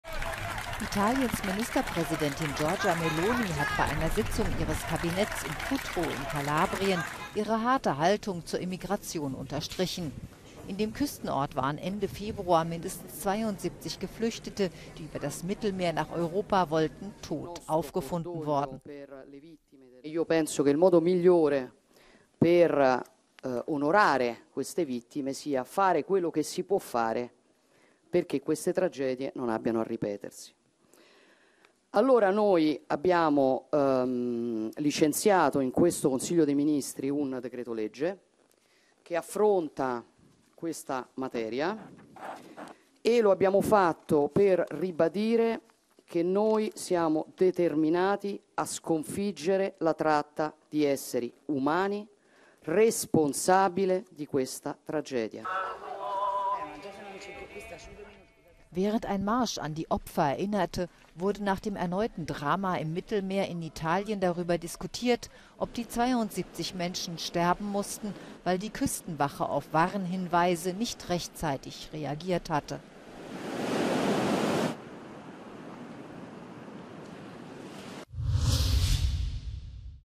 Friedhof Mittelmeer: Meloni bleibt bei harter Haltung in Sachen Immigration
Italiens Ministerpräsidentin Meloni hielt eine Kabinettssitzung in Cutro ab, wo mindestens 72 Geflüchtete ertrunken waren.